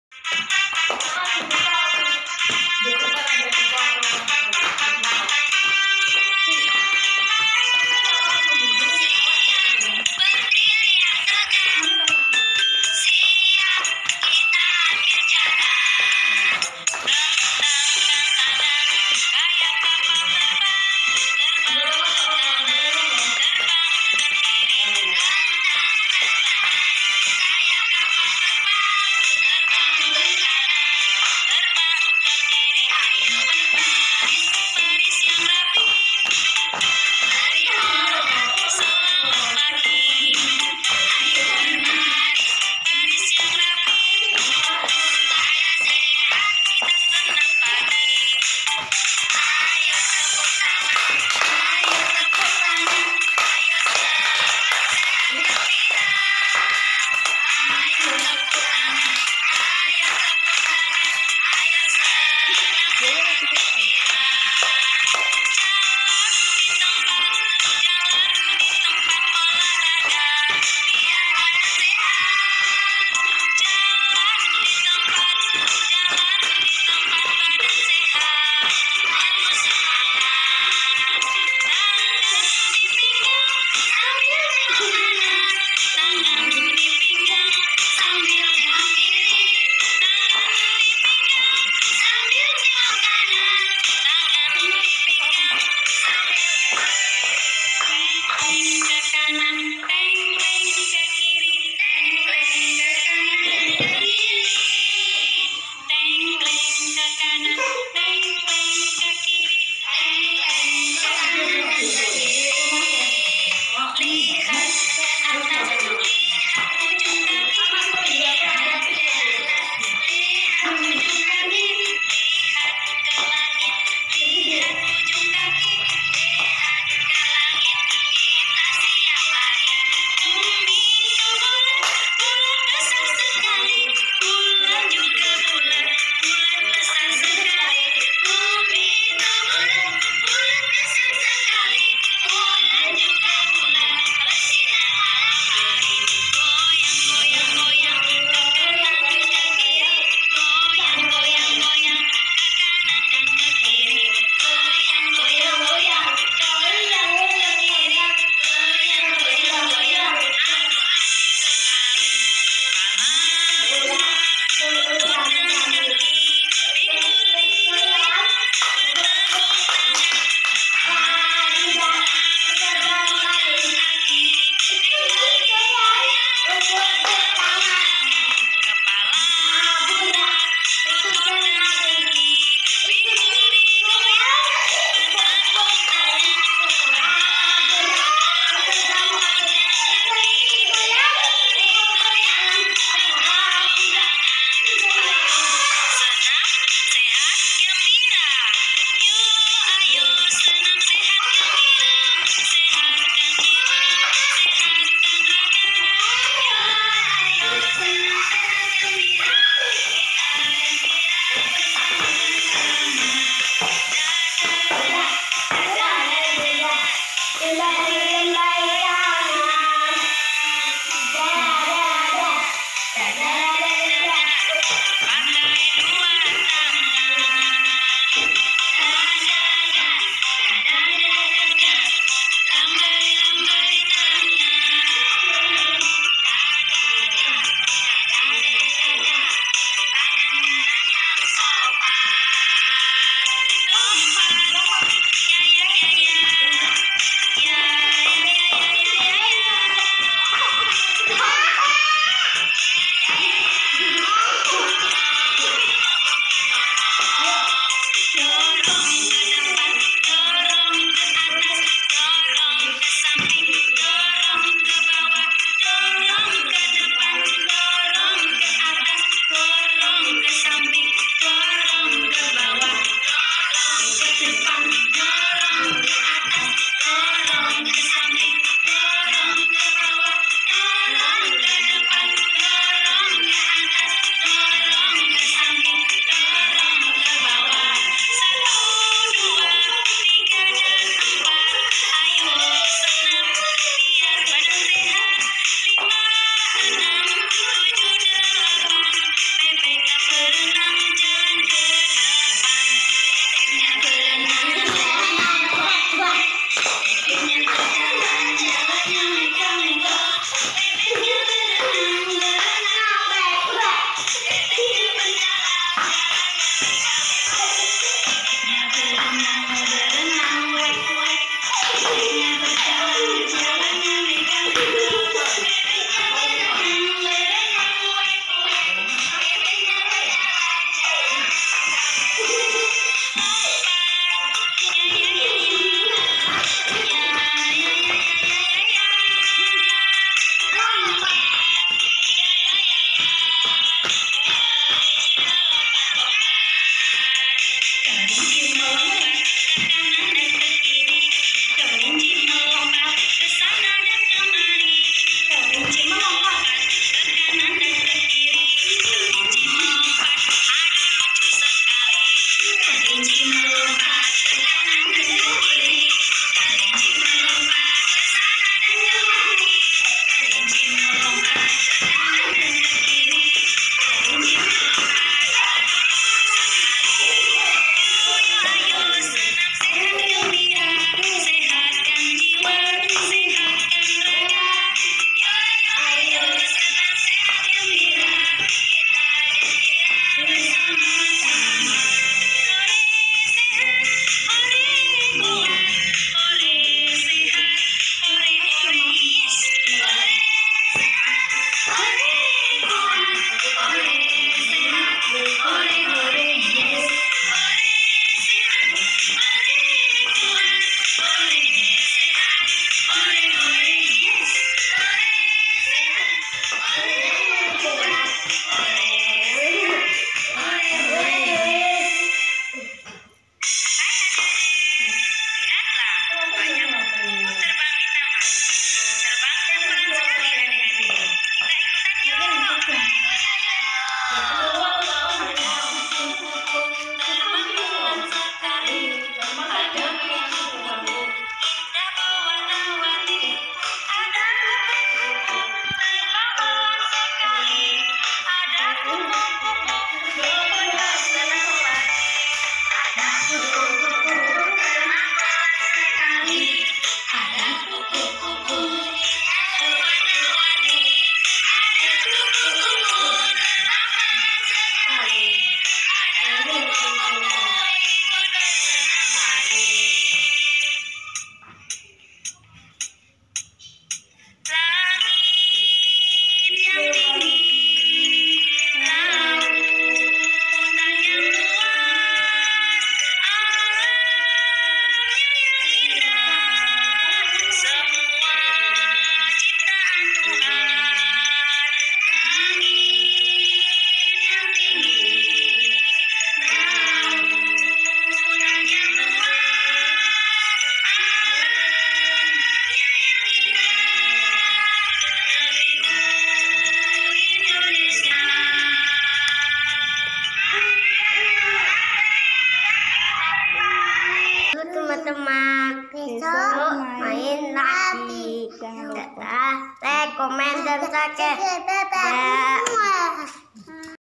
senam anak TK🥰senam sehat ceria
yuk sekali sekali senam biar sehat temen temen